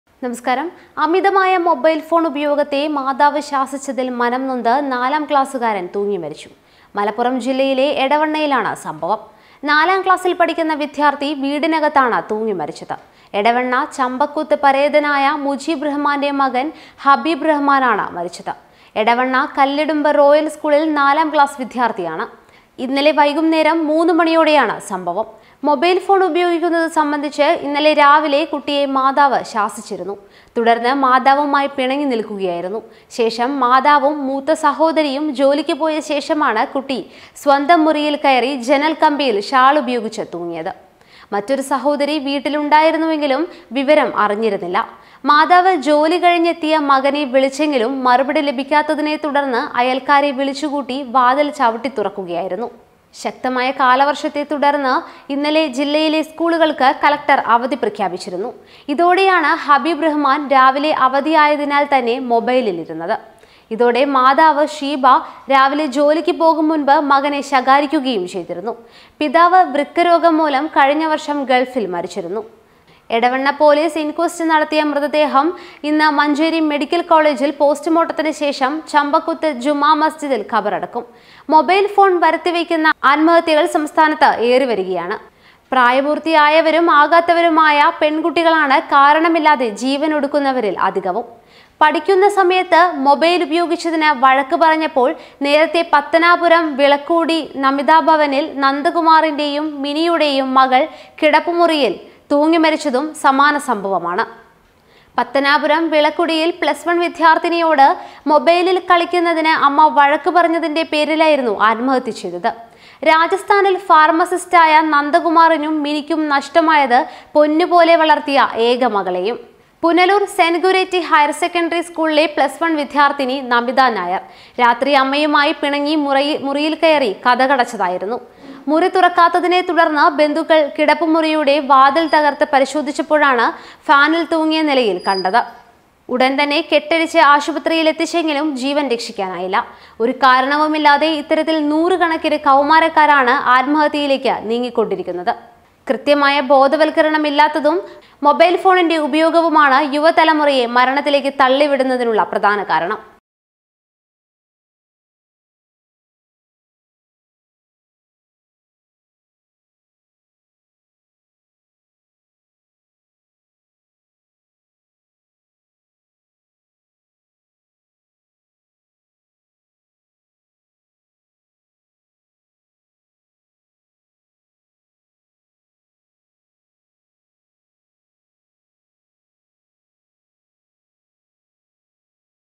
മലപ്പുറത്തെ നാലാംക്ലാസ്സുകാരന്റെ തൂങ്ങി മരണത്തിന് കാരണം ഇങ്ങനെ I mobile phone
സദാസമയം മൊബൈൽ ഫോണിൽ നോക്കിയിരിപ്പ്; വല്ലാതെ കളി കൂടുന്നുവെന്ന് അമ്മ ശാസിച്ചു: മനംനൊന്ത് നാലാം ക്ലാസുകാരൻ തൂങ്ങി മരിച്ചു; കടുംകൈ ചെയ്തത് അമ്മയും മൂത്തസഹോദരിയും ജോലിക്ക് പോയ ശേഷം; സ്വന്തം മുറിയിൽ ജീവനൊടുക്കിയ പതിനൊന്നുകാരന്റെ ദാരുണാന്ത്യത്തിൽ കണ്ണീരൊഴുക്കി വീട്ടുകാരും നാട്ടുകാരും